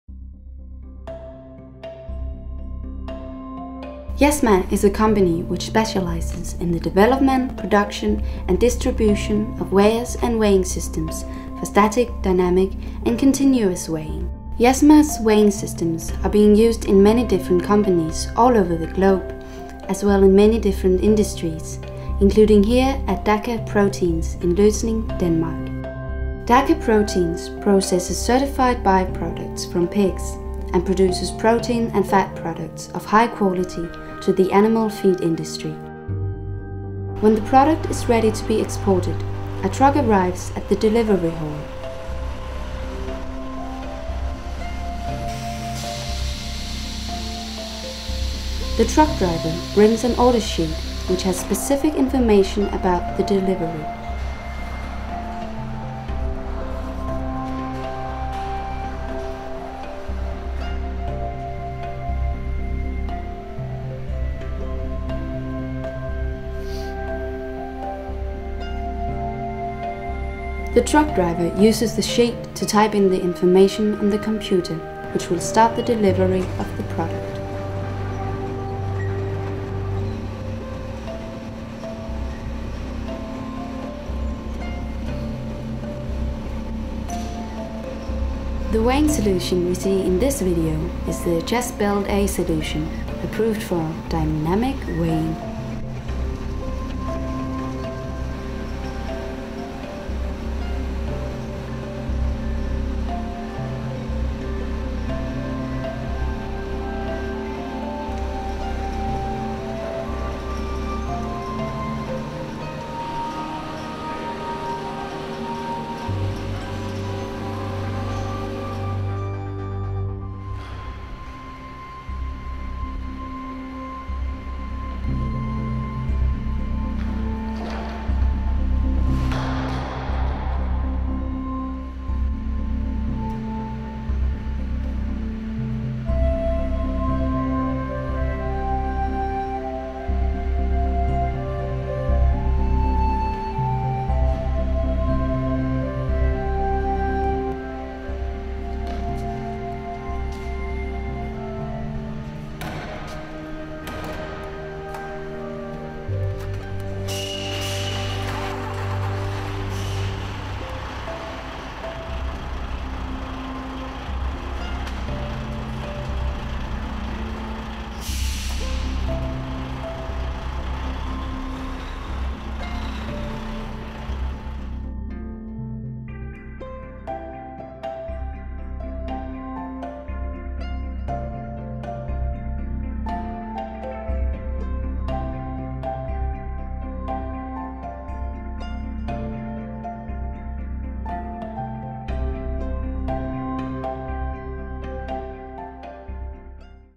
Weigh feeder DAKA Protein
Jesma has delivered an approved weigh feeder solution to DAKA Protein in Denmark.
Many companies still use weigh bridges when the finished product leaves the factory.
To save time and money Jesma can deliver an approved weigh feeder solution for the outloading of the product.